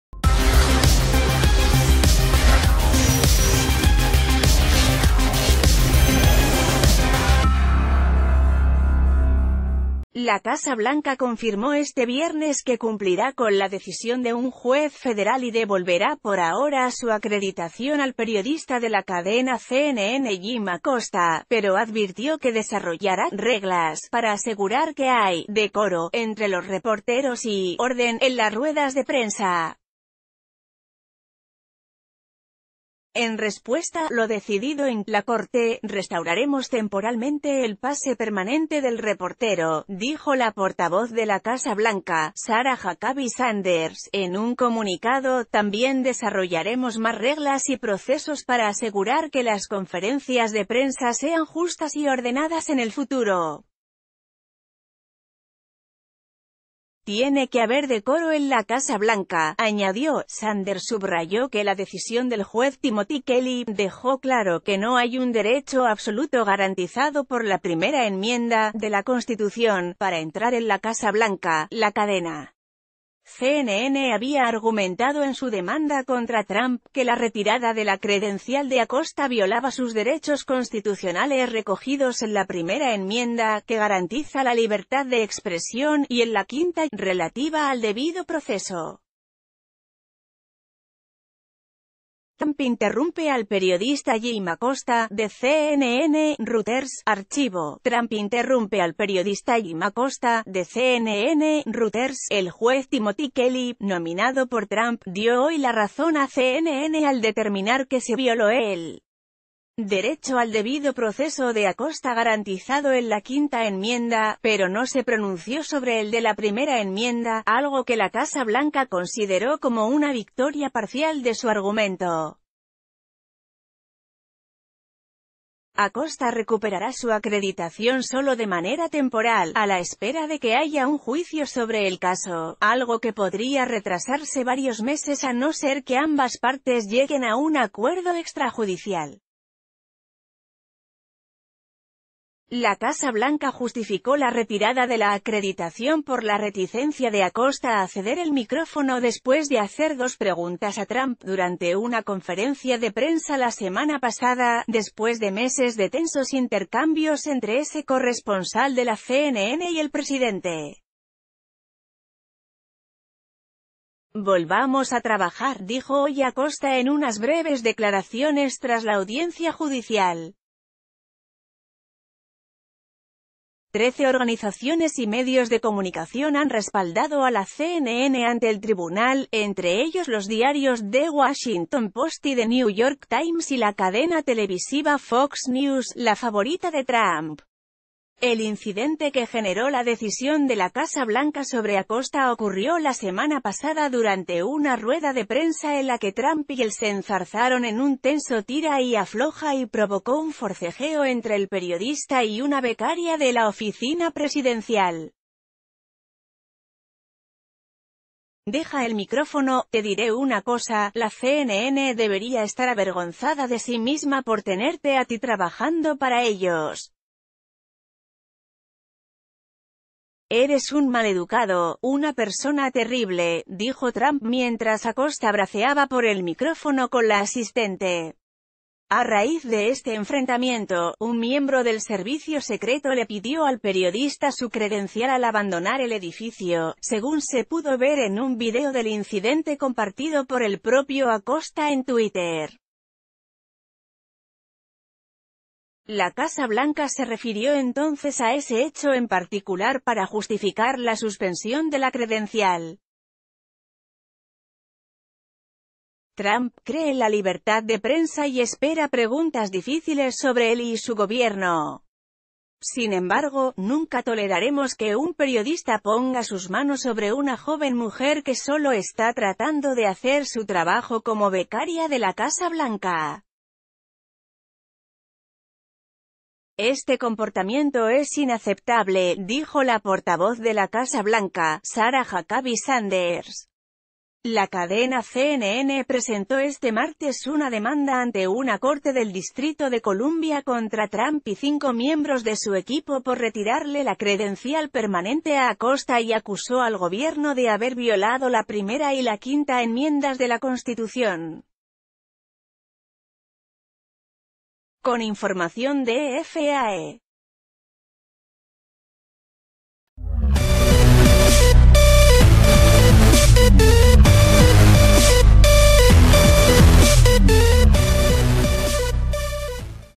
Casa Blanca devuelve acreditación a periodista de CNN, pero exige ‘decoro’
La Casa Blanca confirmó este viernes que cumplirá con la decisión de un juez federal y devolverá por ahora su acreditación al periodista de la cadena CNN Jim Acosta, pero advirtió que desarrollará “reglas” para asegurar que hay “decoro” entre los reporteros y “orden” en las ruedas de prensa. En resp...